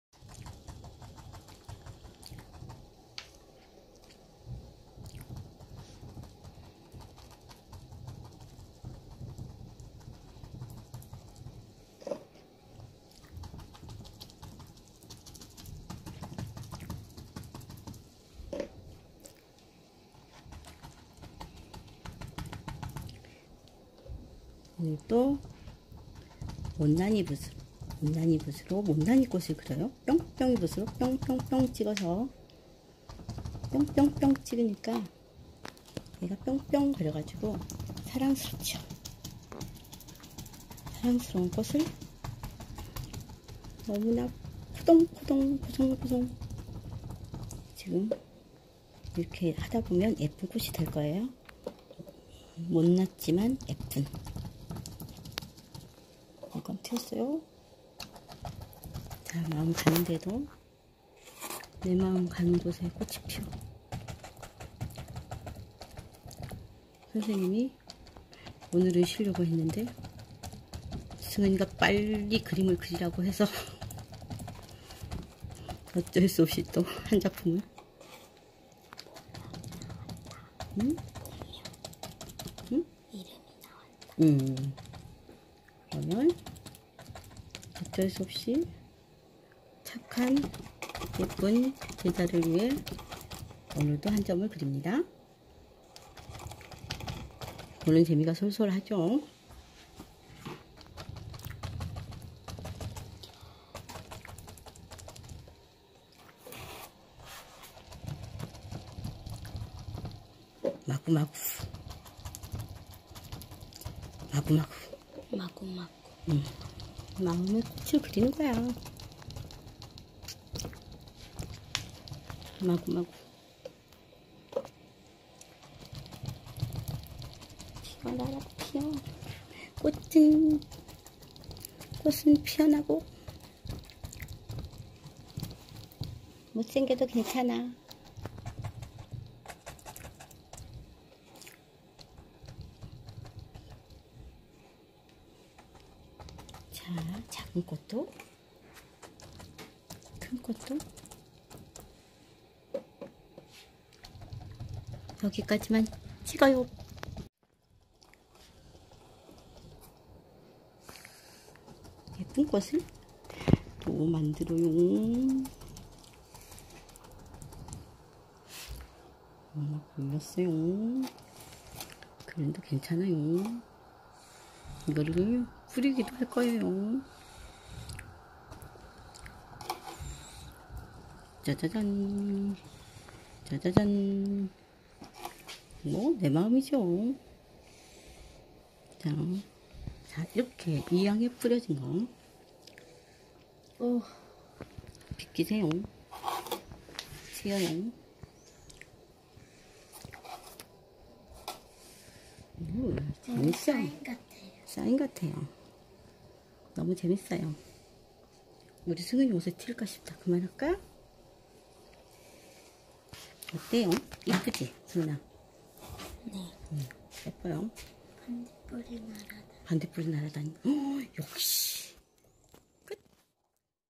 심심풀이 땅콩 마음의 꽃을그려요
뽕뽕이 붓으로 마구마구 찍어서 잼나게 그림그려요~^^♡